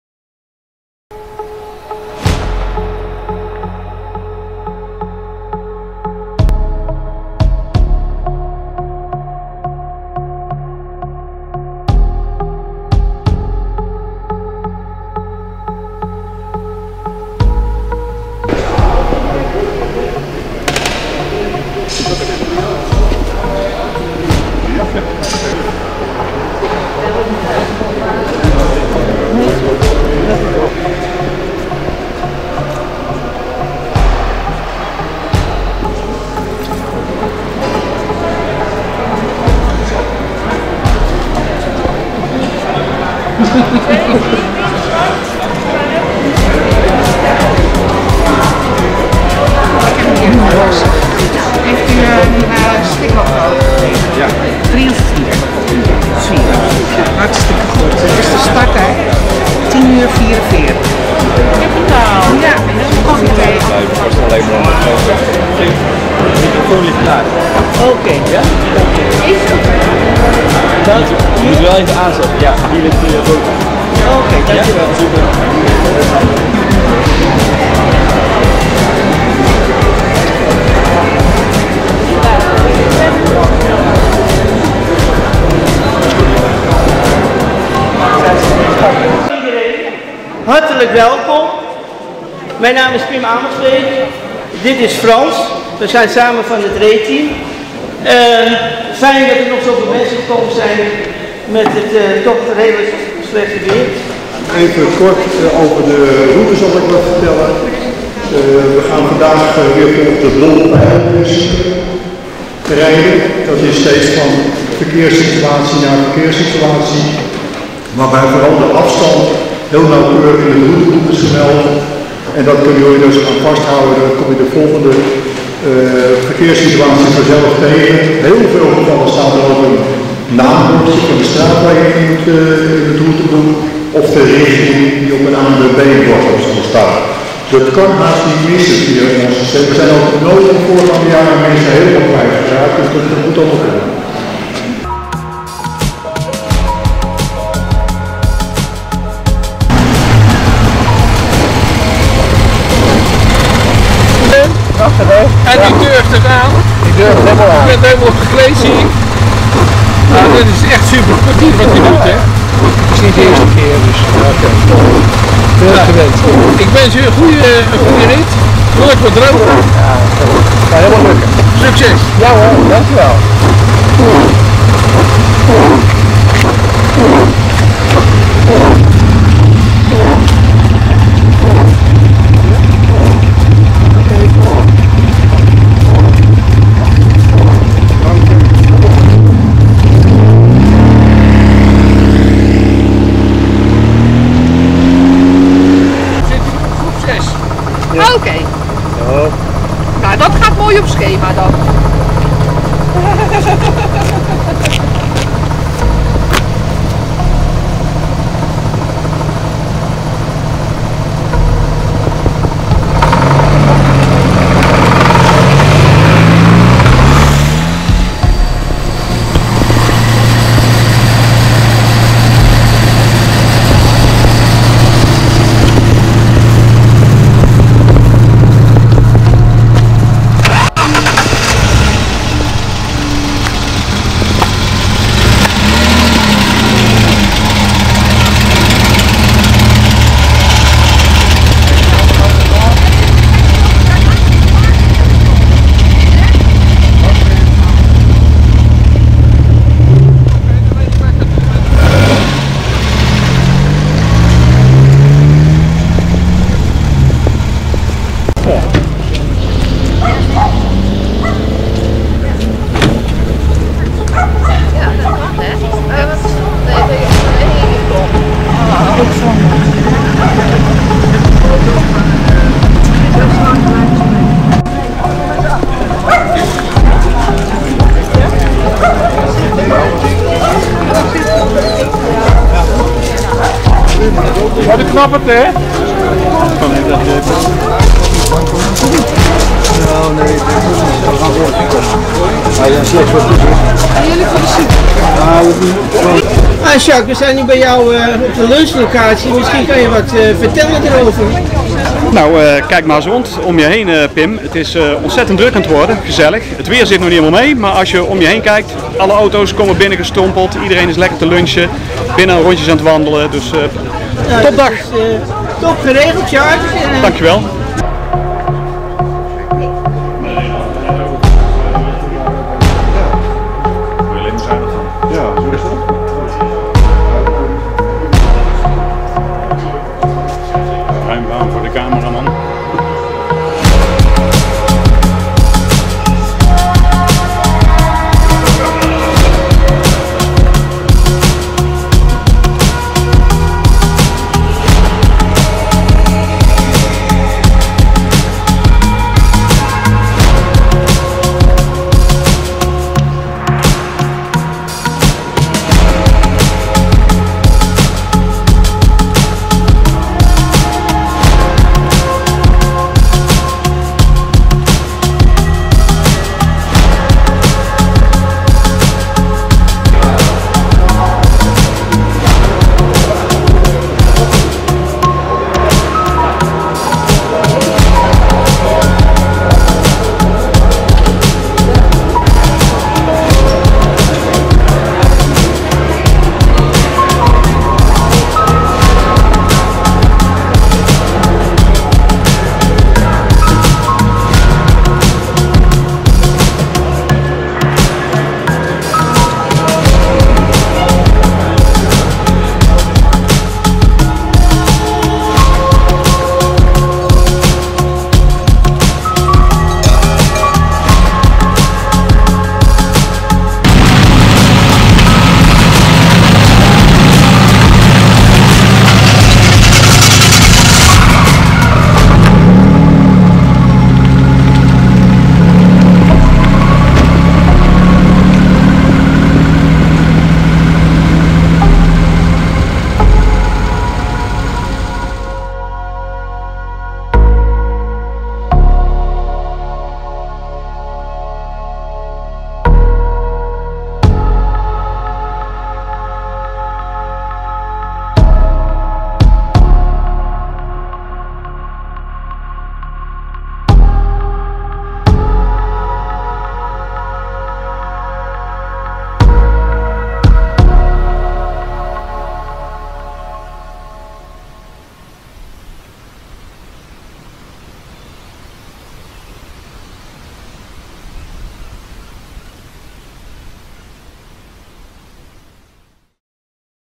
8e REEE 2021 start bij Louwman - Rollend Engels Erfgoed Evenement
8e REEE 2021 tourrit op zondag 3 oktober

Start bij Louwman Toyota World in Raamsdonksveer
Lunch bij Loco Parts in Oirschot
Finish bij De Gruyter in 's-Hertogenbosch

Een REEE-TV Productie
Pim Amelsbeek - film en montage
Hans Köhler - regie